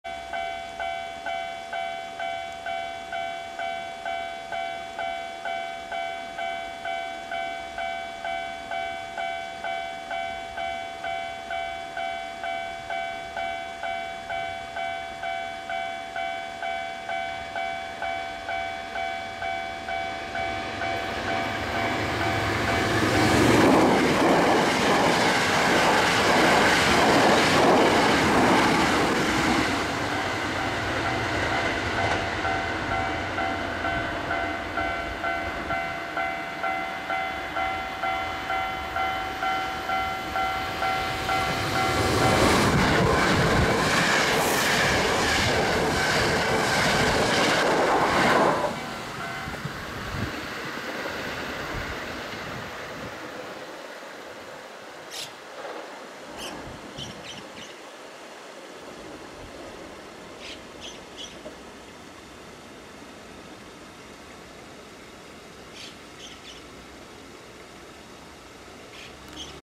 阪急京都線西山天王山～大山崎間を走行する9300系の快速梅田行きと普通河原町行き
2016年4月20日（水）撮影。阪急京都線西山天王山～大山崎間の踏切で撮影した、9300系9302Fの快速梅田行きと9305Fの普通河原町行きの走行シーンです。